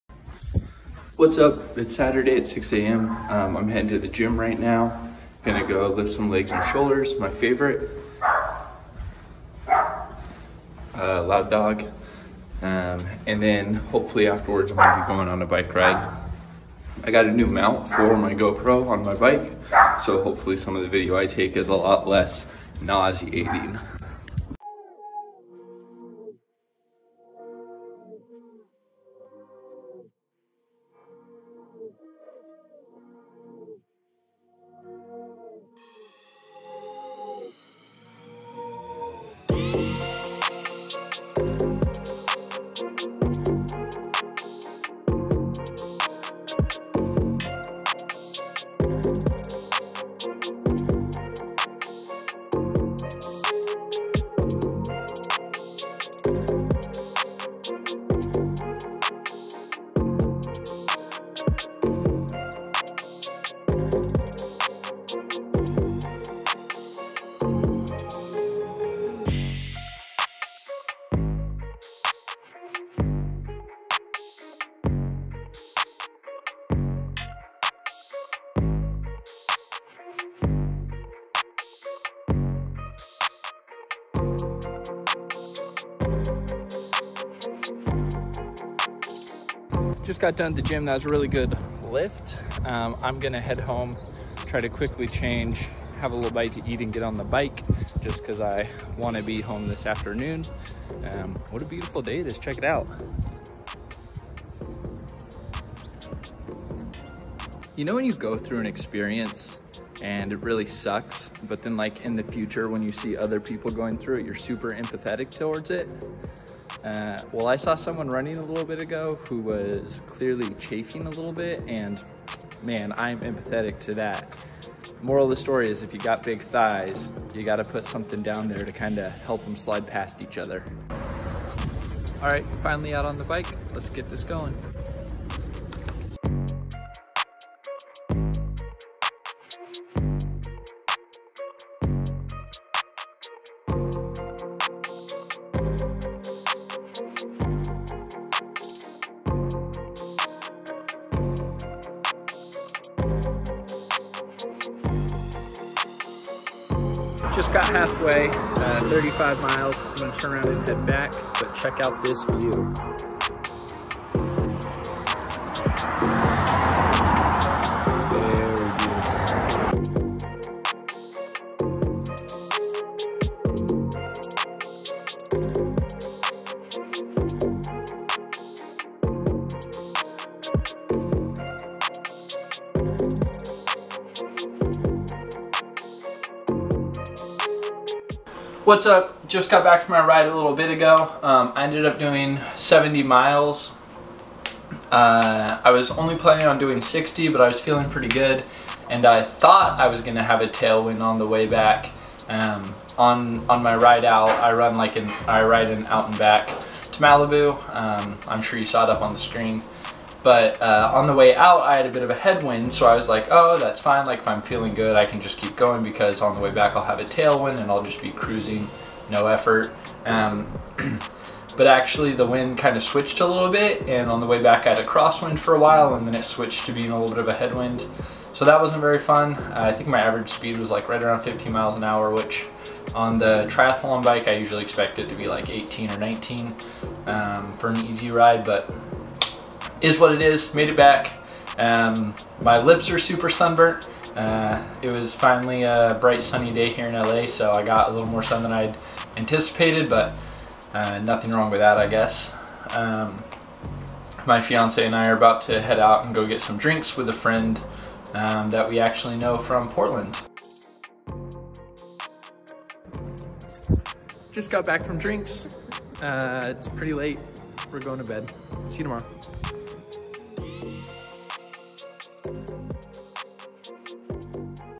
Long PCH Ride
70 Mile out and back up the PCH